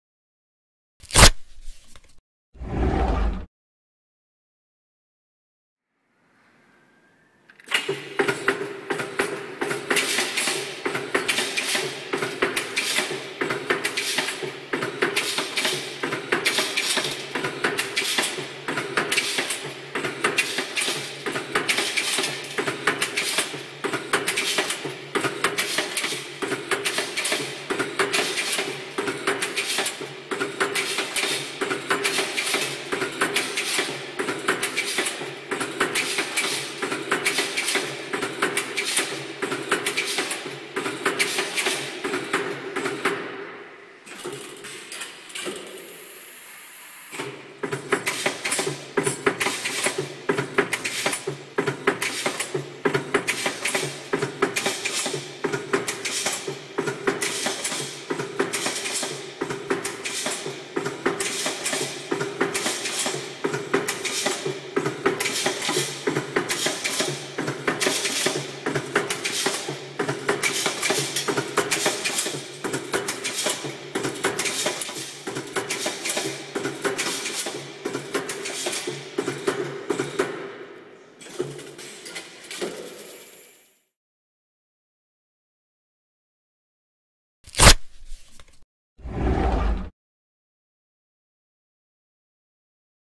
Ball Bearing Insertion System - Altec Engineering Ltd
The challenges for Altec Engineering Ltd on this project included inserting ball bearings into a ball carrier and feeding the assembled carriage into an outer rail assembly to complete the process.

The machine was built upon a robust steel fabricated framework with a steel base plate and an aluminium extrusion upper guard frame with clear polycarbonate panels. The machines electrical control and pneumatic components were housed in a cabinet at the rear of the framework.